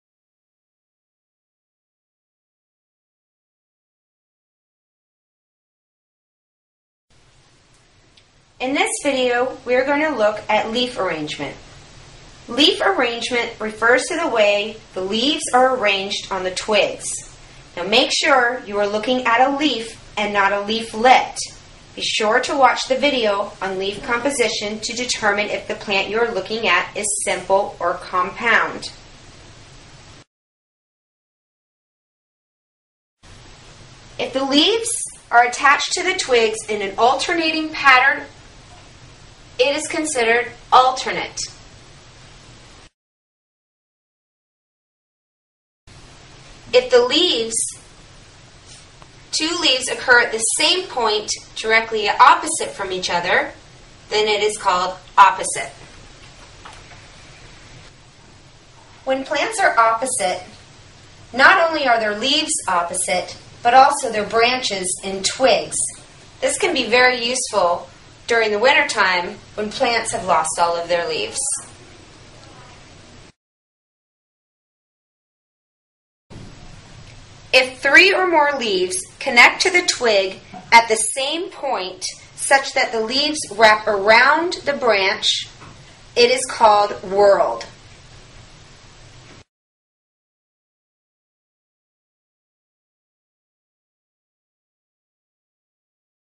Leaf Arrangement
Tree identification is mostly based on leaves! Increase your tree identification skills by learning how to distinguish between different types of leaf arrangement.